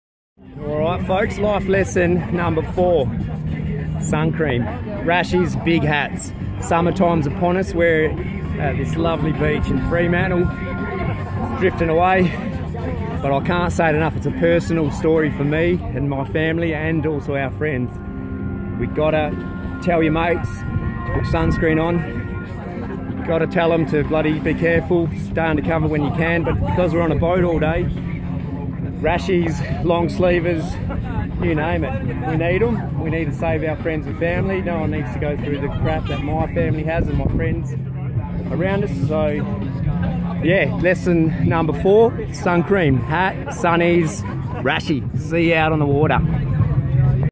Get sun smart as cancer is a bitch.
SUMMER time is close down under in Australia.
Be sunsmart, wear long sleeved shirt, wear hats, sunglasses, suncream 30+ or more
Seek shade when you can. .

Be a hero and save your life and your friends life